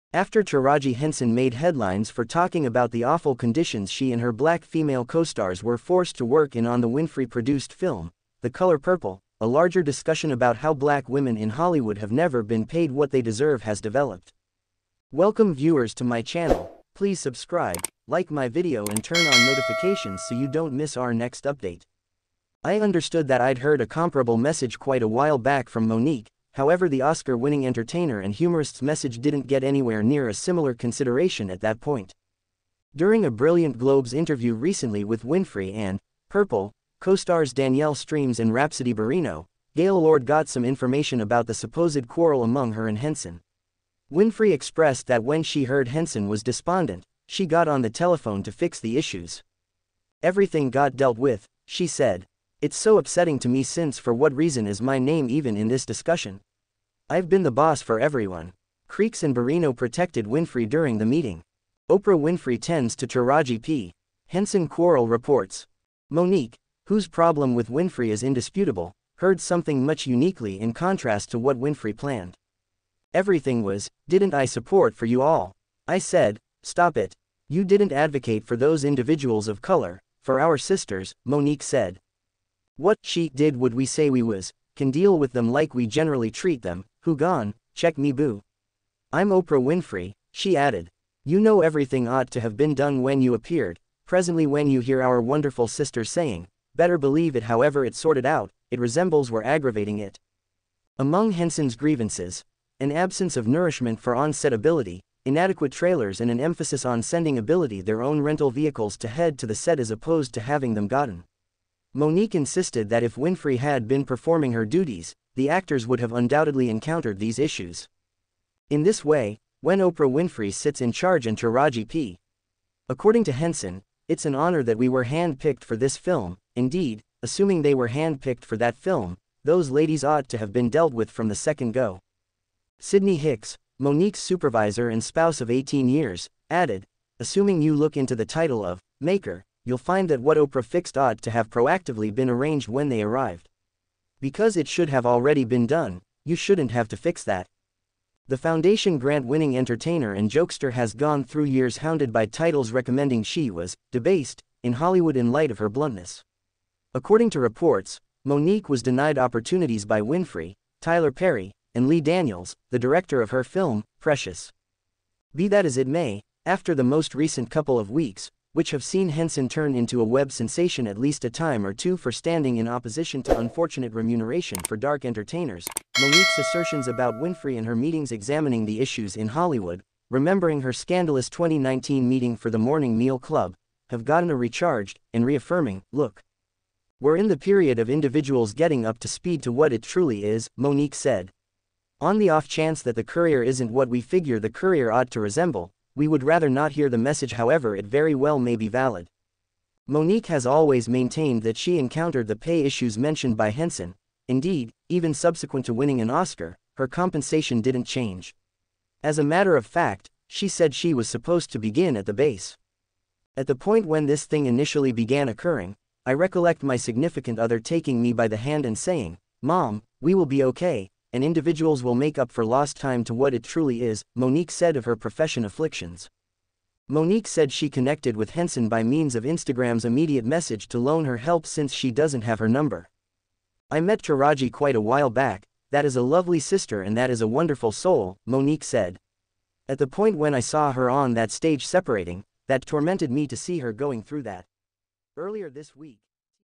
Monique jump in on Taraji Henson Oprah fig:ht to expose what Oprah does to female black actors
tarajiphenson tarajihenson oprah oprahwinfrey tarajiphensonoprahAfter Taraji Henson made headlines for talking about the awful conditions she and her Black female co-stars were forced to work in on the Winfrey-produced film "The Color Purple," a larger discussion about how Black women in Hollywood have never been paid what they deserve has developed.

I understood that I'd heard a comparable message quite a while back from Mo'Nique, however the Oscar-winning entertainer and humorist's message didn't get anywhere near a similar consideration at that point